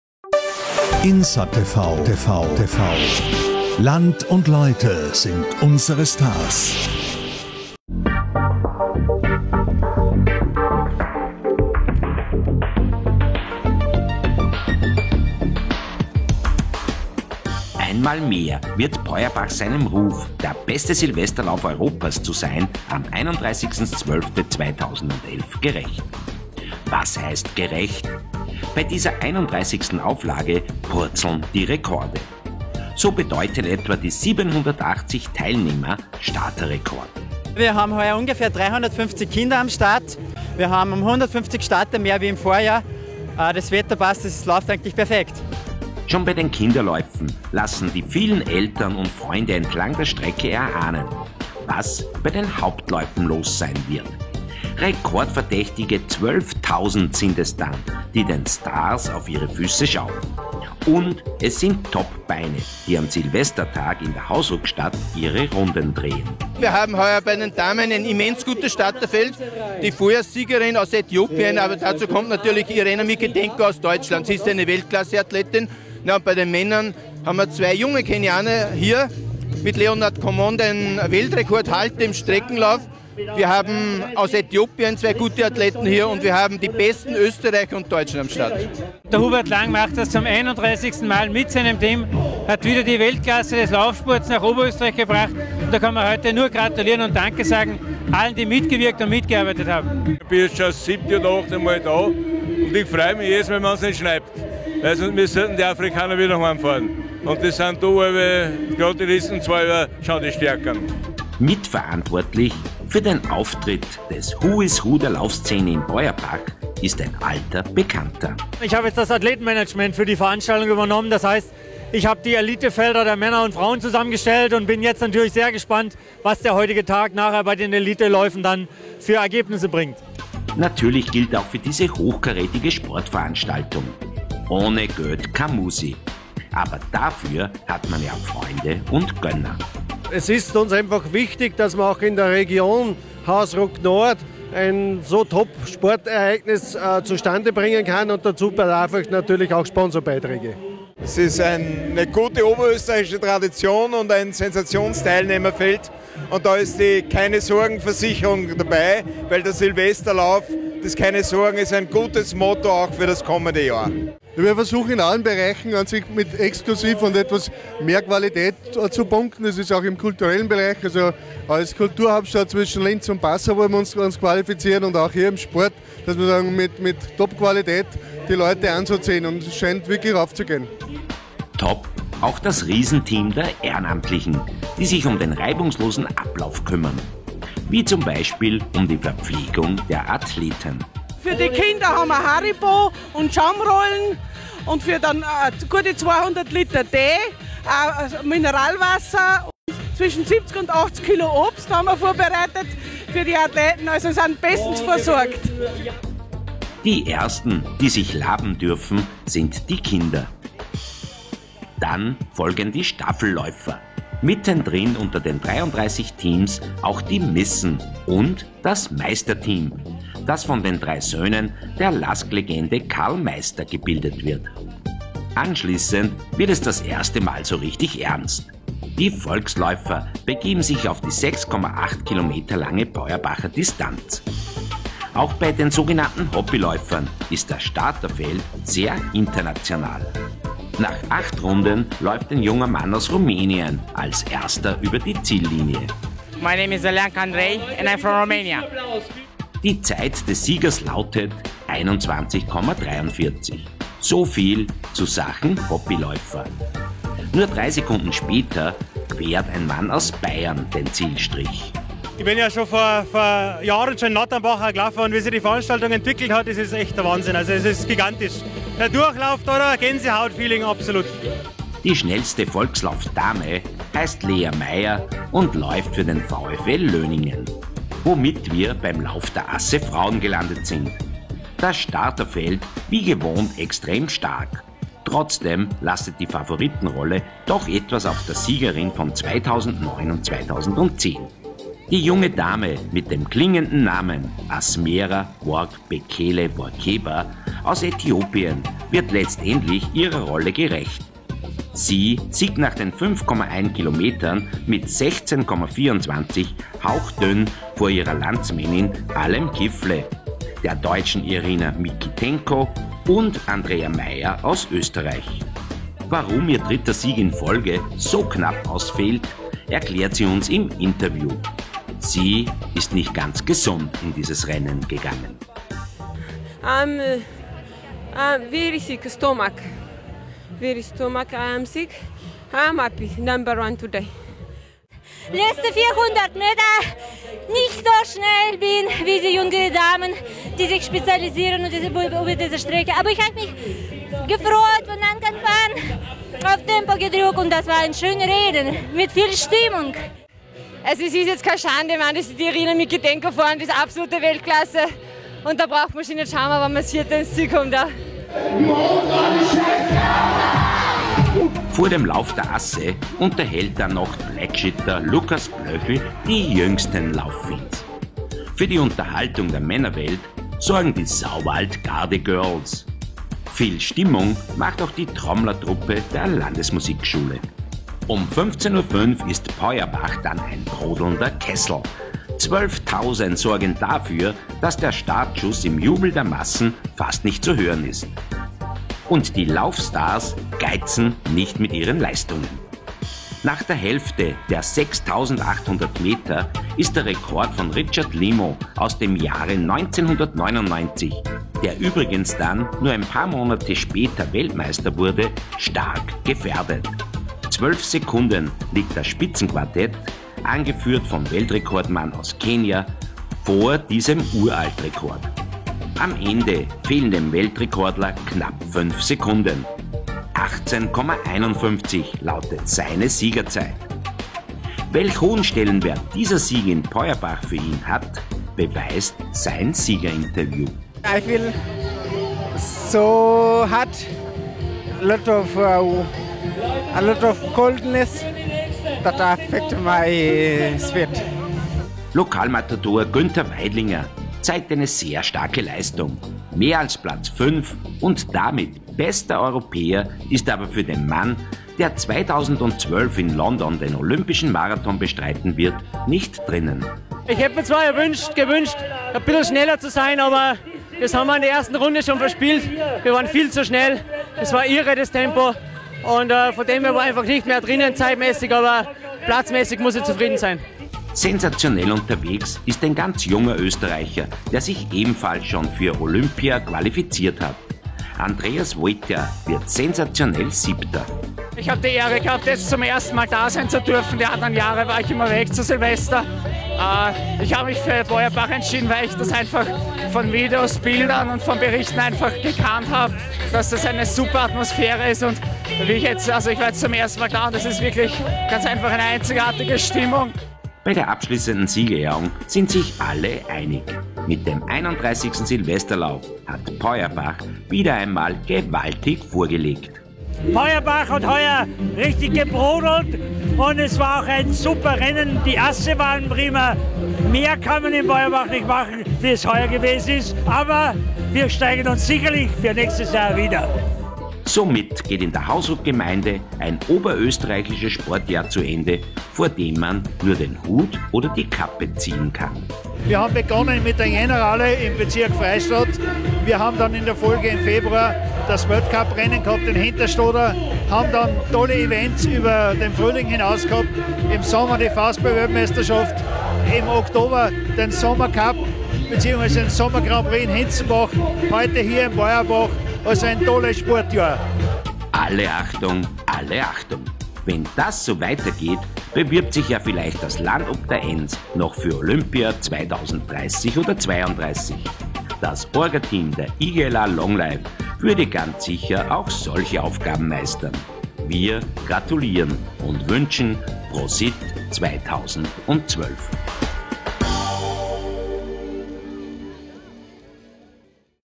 Peuerbacher Silvesterlauf 2011
Peuerbach schafft es Jahr für Jahr den Silvesterlauf als „sportlichen Höhepunkt" zu absolvieren. Am letzten Tag des Jahres 2011 sorgte Weltrekord-Mann Leonard Komon (Foto) und die Äthiopierin Asmera-Work-Bekele Workeba für Favoritensiege.
welsin.tv war mit der Kamera dabei und zeitgt im Filmbeitrag den Vergleich zu Wels, wo in erster Linie aus Freude am Laufsport gelaufen wird.
Der Peuerbacher Silvesterlauf lebt von der Prominenz im Starterfeld, was in Wels für die Veranstalter frustrierend wäre. Denn die Welser sind nur schwer „hinter dem Ofen hervorzubringen" ist schwierig. Die Peuerbacher leben von der Begeisterung der Besucher.
Der erste große Sieg des Tages ging an die Äthiopierin, Workeba gewann in 16:24,2 min. den „Lauf der Asse" der Frauen, der heuer erstmals getrennt von ihren männlichen Kollegen ausgetragen worden war. Die Favoritin schaffte damit den ersehnten Hattrick, denn die 20-Jährige siegte bereits auch 2009 und 2010 in Peuerbach.
Anschließend folgte der Asselauf der Männer als  Höhepunkt des Tages. Weltrekordhalter Leonard Komon aus Kenia, kam, sah und siegte: Der 23-jährige dominierte das acht Runden dauernde Rennen, in 18:51,0 Minuten verpasste Komon den Uralt-Rekord aus dem Jahr 1999 (18:46,09) jedoch knapp. Zweiter wurde Juniorenweltmeister Josphat Bett aus Kenia (18:58,1) vor dem Äthiopier Millyon Zewdie (18:59,5).
Viel Applaus spendeten die euphorisch jubelnden Zuschauer jedoch nicht nur den Afrikanern, sondern nach jeder Runde auch Günther Weidlinger, als siebenfacher Silvesterlaufsieger schließlich immer noch der Publikumsliebling in Peuerbach. Weidlinger landete hinter dem Äthipier Zelalem-Bach Regasa (19:22,3 Minuten) auf Rang 5 (19:23,1 Minuten). Als zweitbester Österreicher lief Andreas Vojta als Siebenter über die Ziellinie, Christian Pflügl vom Veranstalterverein Sportunion IGLA long life wurde Zwölfter.
Das Nationen-Duell zwischen Österreich und Deutschland, das im Asselauf der Männer und Frauen ausgetragen worden war, ging mit 47 zu 48 Punkten denkbar knapp an Deutschland.
Gewertet wurden dafür jeweils die drei Schnellsten der beiden Nationen, deren Platzierungen als Punkte addiert wurden. Das Publikum feuerte die Athleten mit Fähnchen und Klopfschläuchen in den jeweiligen Nationalfarben lautstark an.
Für Stimmung sorgte heuer in Peuerbach „Traktor-Gangsta" Lukas Plöchl mit seinen drei Live-Auftritten ebenso wie die „Sauwald-Gardegirls" und eine Trommlergruppe.
Insgesamt waren in Peuerbach heuer 780 Teilnehmer laufend unterwegs, das bedeutet neuen Teilnehmerrekord. Das milde Wetter -- das Rennen fand bei plus zwei Grad Celsius statt -- lockte auch eine Rekordmenge an Zuschauern in das Stadtzentrum, mehr als 10.000 sportbegeisterte Menschen machten die kleine Stadt im Hausruckviertel am letzten Tag des Jahres 2011 zu einem „Mekka des Laufsports".